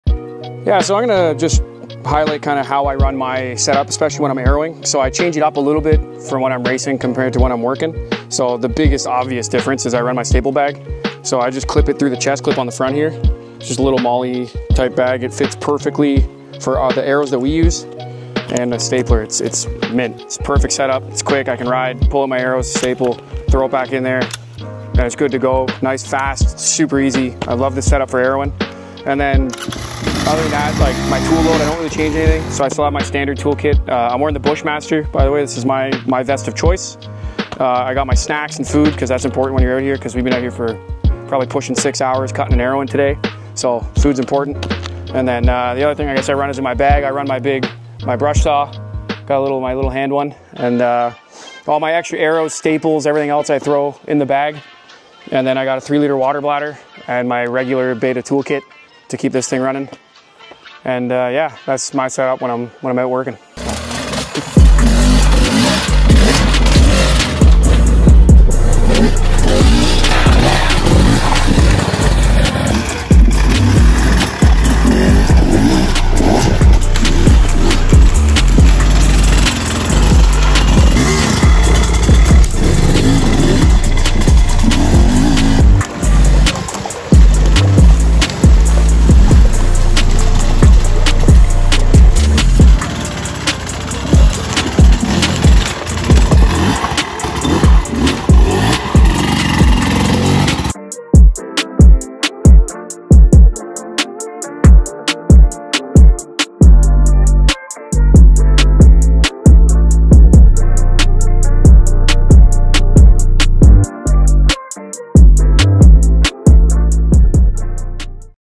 Our TekVest Bushmaster Setup when Arrowing and Maintaining Trails
Kevin is here to show you how he sets up his TekVest Bushmaster for maintaining the Corduroy Enduro Trails.